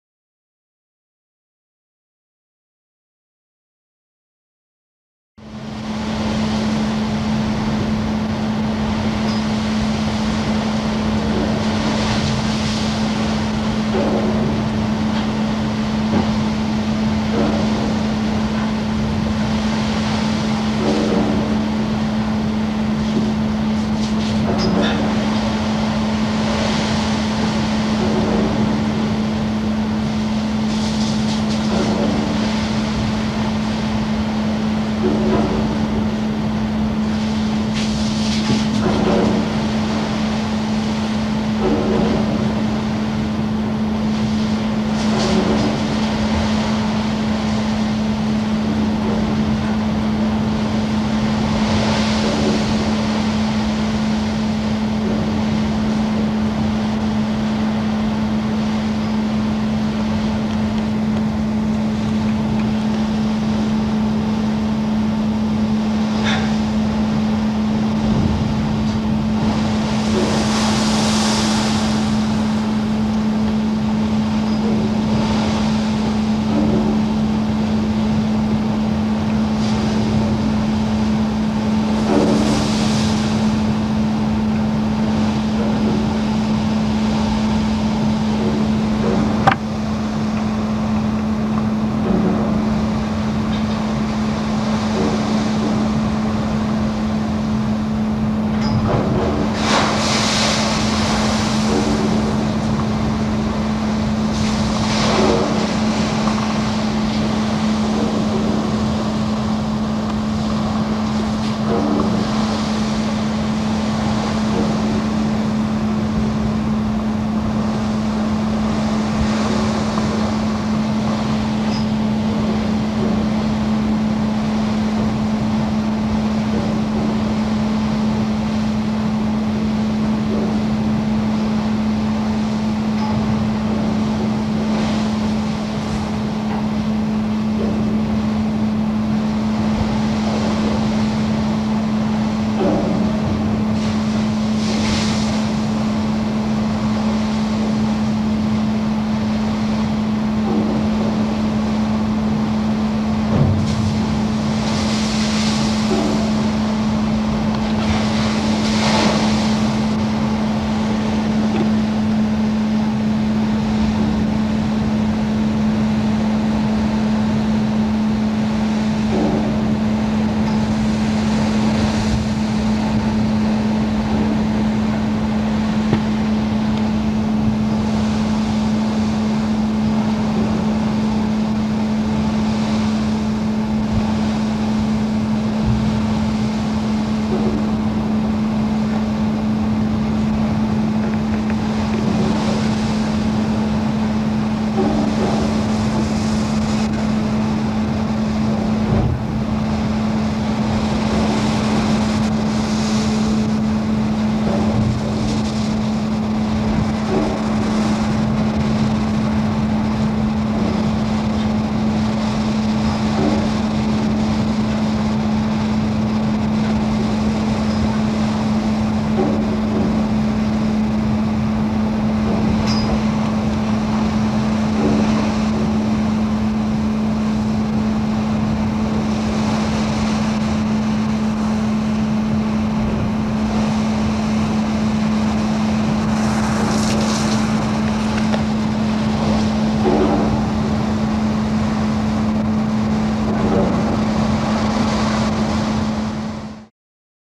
5 Seasons Book video clip 31 - Ryerson waves on Superior
S/S Edward L Ryerson taking waves on Lake Superior 1993  - Five Seasons on Steel Decks book companion video.